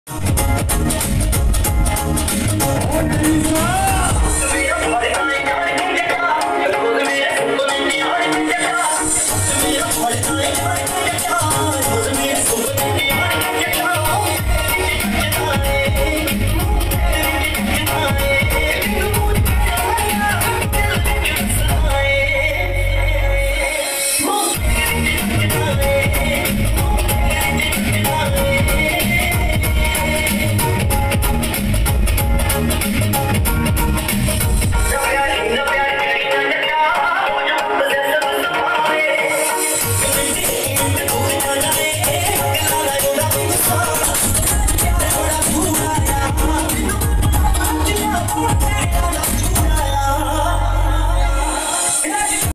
Bhangra 20023 by punjabi kids
viral cute kids performing bhangra at stage in punjab, @walkandwatch